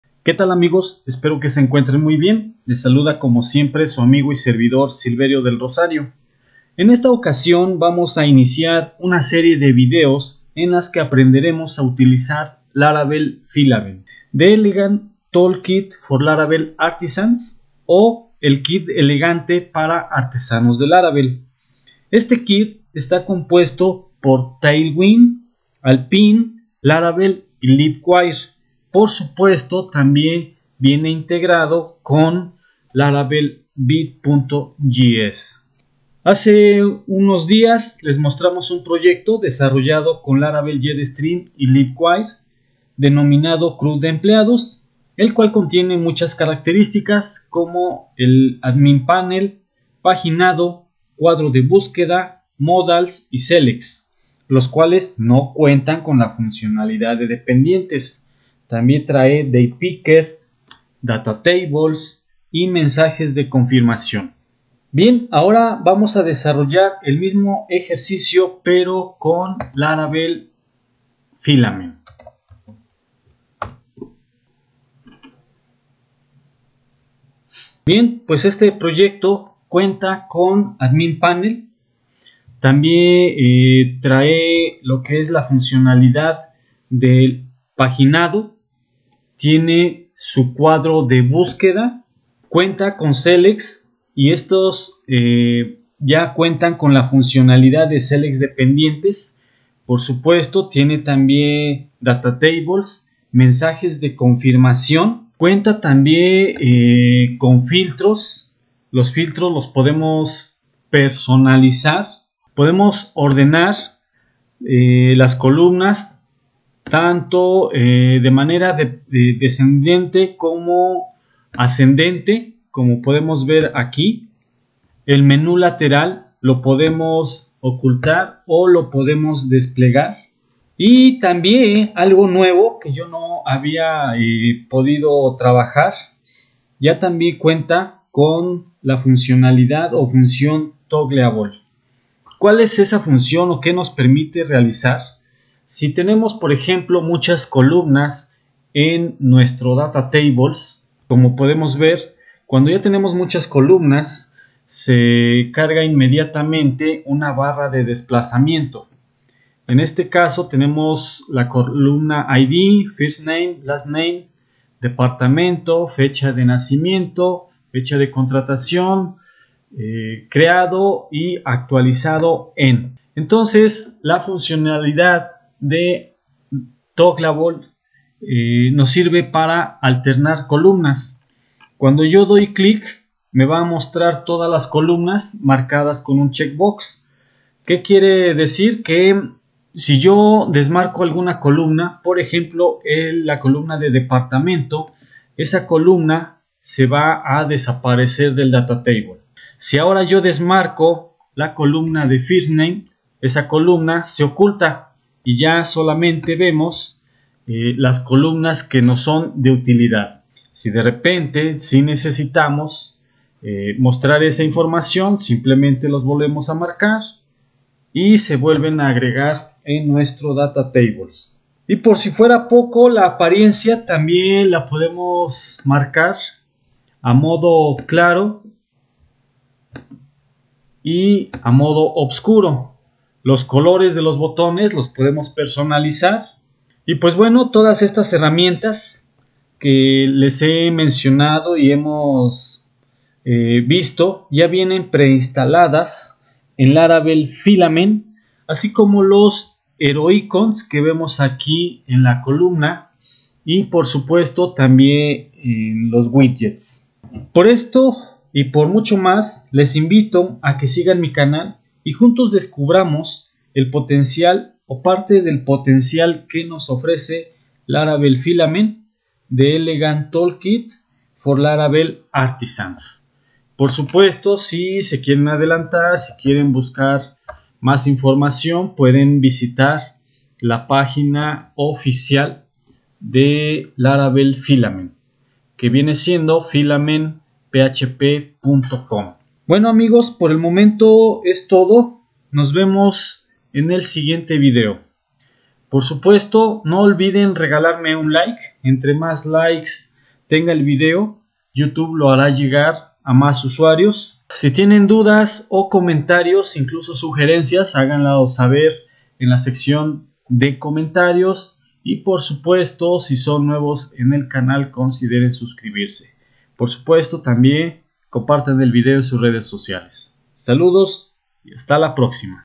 💻Laravel Filament con Vite JS, 🚨Desarrollamos un crud y aprendemos a utilizar todas sus herramientas
Iniciamos una seria de videos en la que aprenderemos a utilizar el nuevo kit de desarrollo para artesanos de Laravel, y me refiero nada más y nada menos que Laravel Filament.

Utilizaremos el admin panel, datatables, con paginado, cuadro de busqueda, filtros, selects, estos mismos con la funcionalidad de selects dependientes, también vemos los mensajes de confirmación que ya vienen pre-diseñados, las columnas del datatable se pueden ordenar tanto en descendente como en ascendente, el menu lateral lo podemos ocultar y mostrar, y la apariencia de la app web la podemos cambiar a modo día o modo noche.

Laravel Filament ya contiene heroicons y widgets pre-definidos, listos para ser utilizados.

Marcas de tiempo:

00:00 Inicio